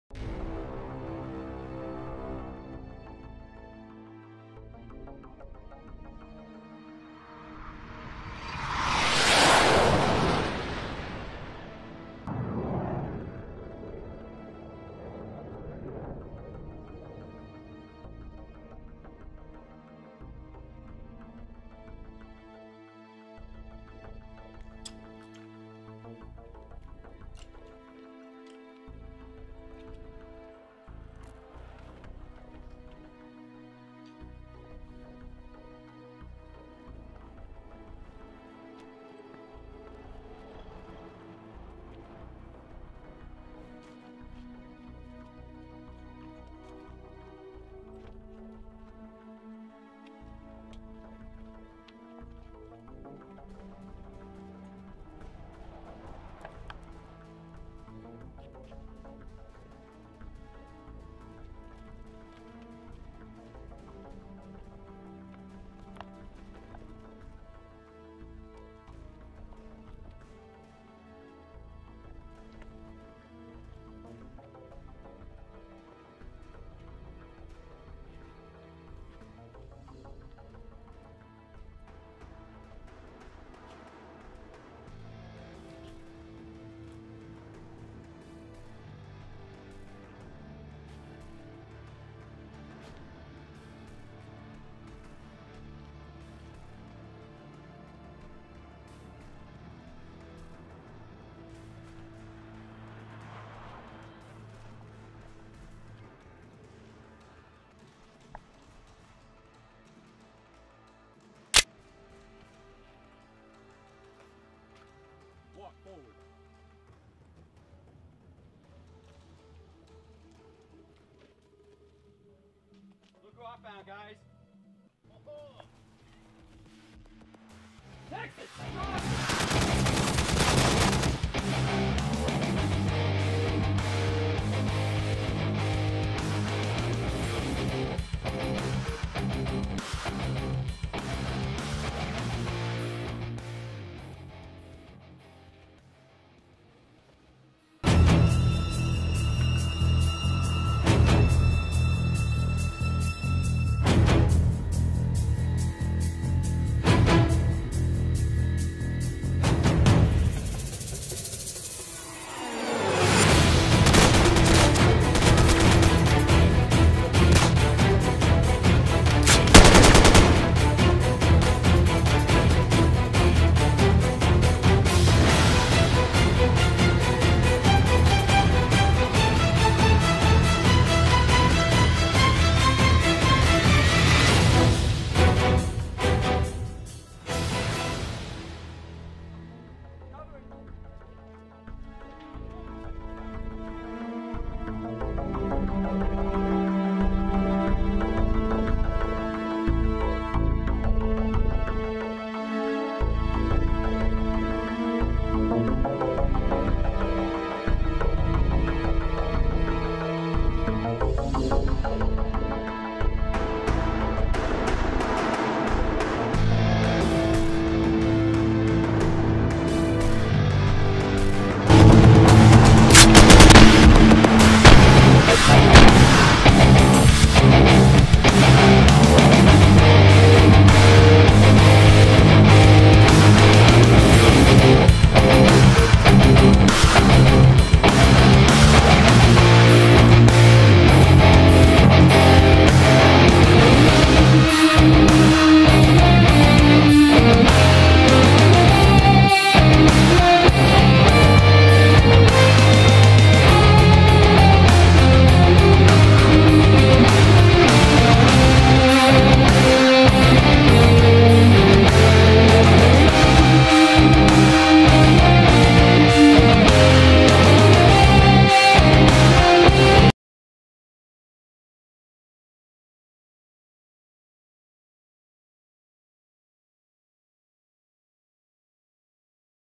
VFX Practice #6 | Short Film #2
Short Film practice test filmed on location, Black Oak,AR.     NOTE: This film is just for fun and practice and is not to be taken seriously, all extras that were seen in the film were on complete accident such as reflections and so on.